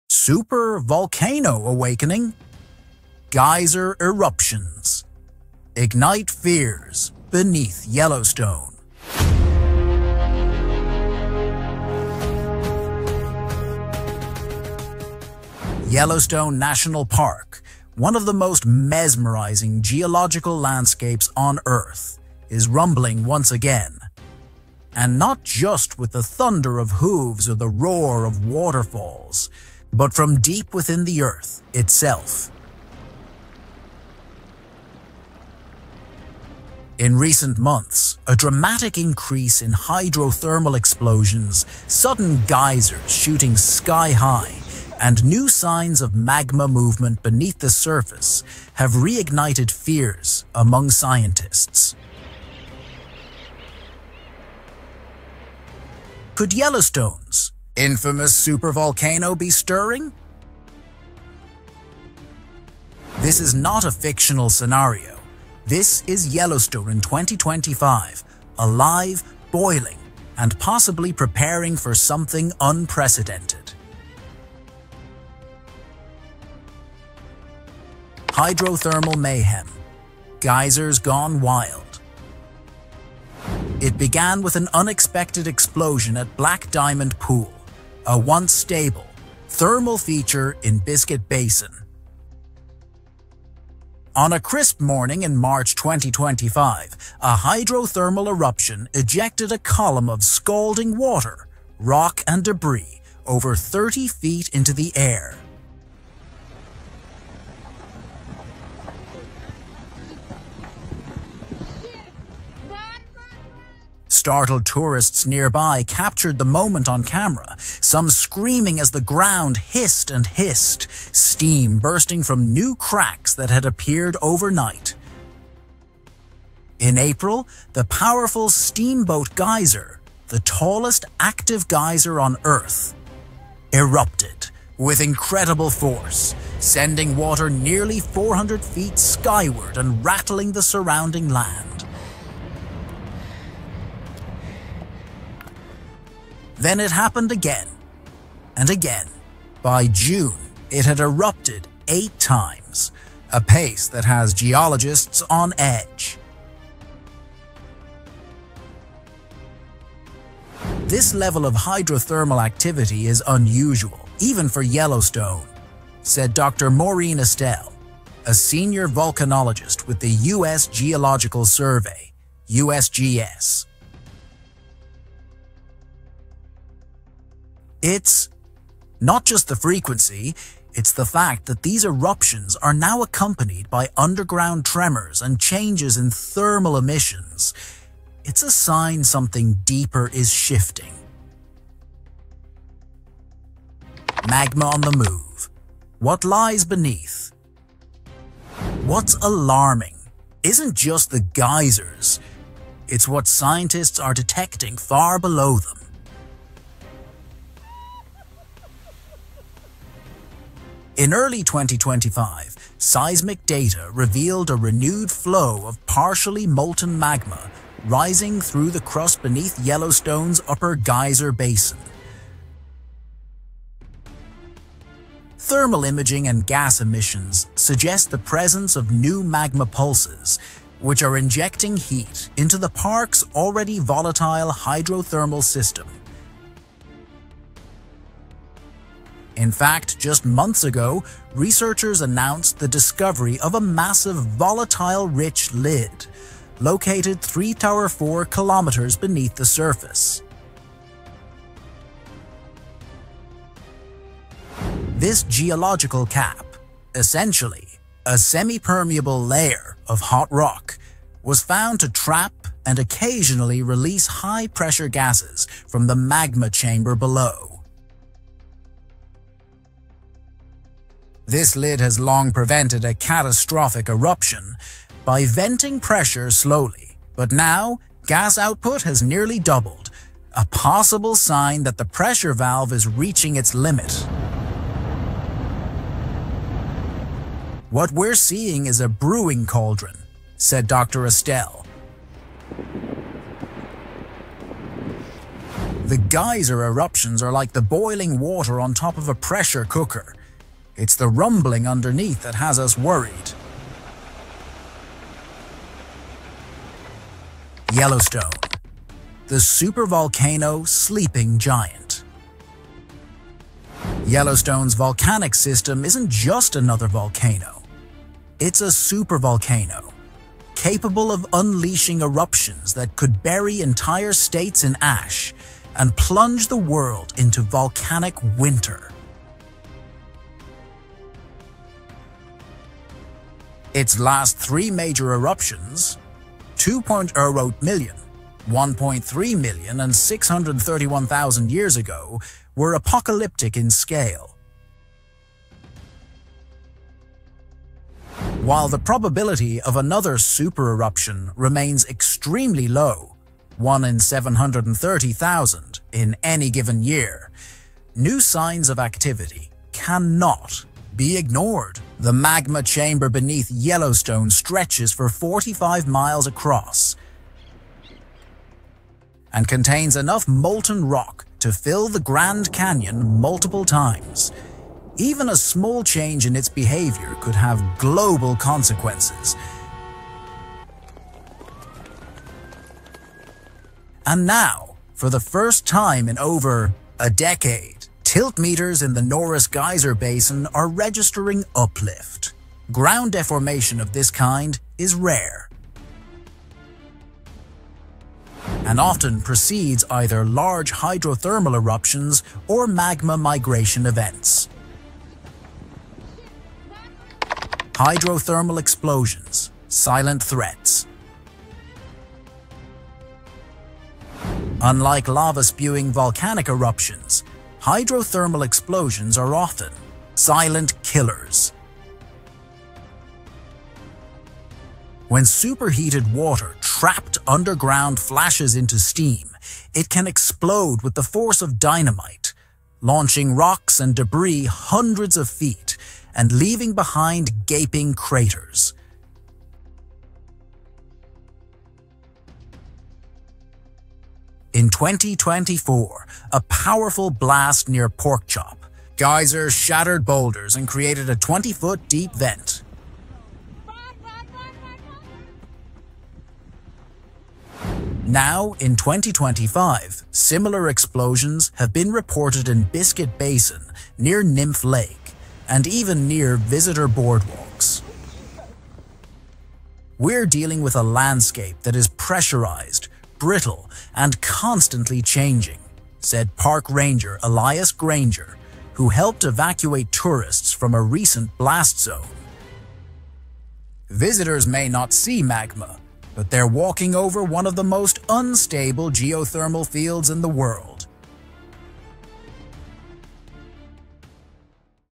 Huge Eruption in Yellowstone! Horrific Camera of Supervolcano Erupts Under Earth Crust! USGS Alert
Huge Eruption in Yellowstone! Horrific Camera of Nat ure's Fury Etna di sicily Volcano Powerfully Eruption

🌋 Supervolcano Awakening? Geyser Eruptions Ignite Fears Beneath Yellowstone
Yellowstone National Park, one of the most mesmerizing geological landscapes on Earth, is rumbling once again—and not just with the thunder of hooves or the roar of waterfalls, but from deep within the Earth itself. In recent months, a dramatic increase in hydrothermal explosions, sudden geysers shooting sky-high, and new signs of magma movement beneath the surface have reignited fears among scientists: Could Yellowstone’s infamous supervolcano be stirring?

This is not a fictional scenario. This is Yellowstone in 2025—alive, boiling, and possibly preparing for something unprecedented.